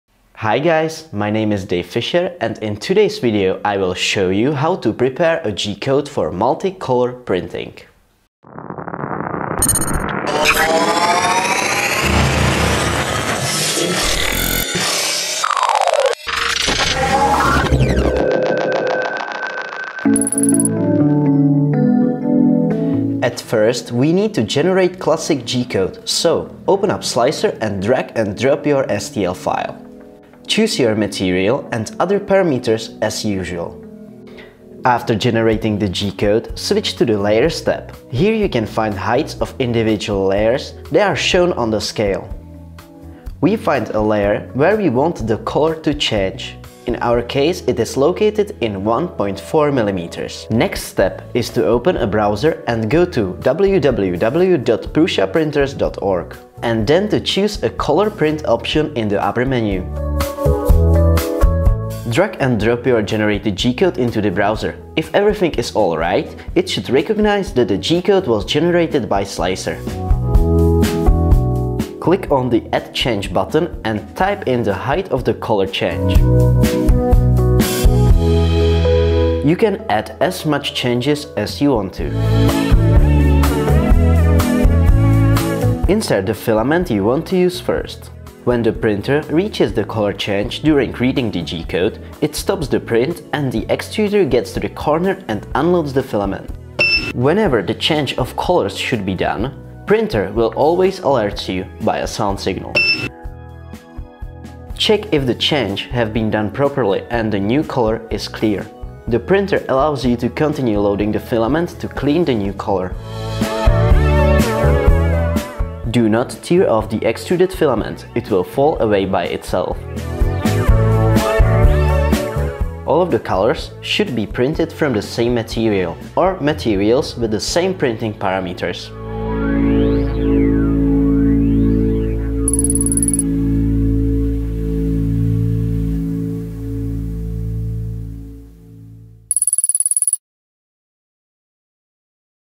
Multi-Color 3D Printing
How to print in multiple colors on the Original Prusa i3? This video shows you the entire process!

My name is Dave and I am helping Josef with YouTube videos when he is busy :)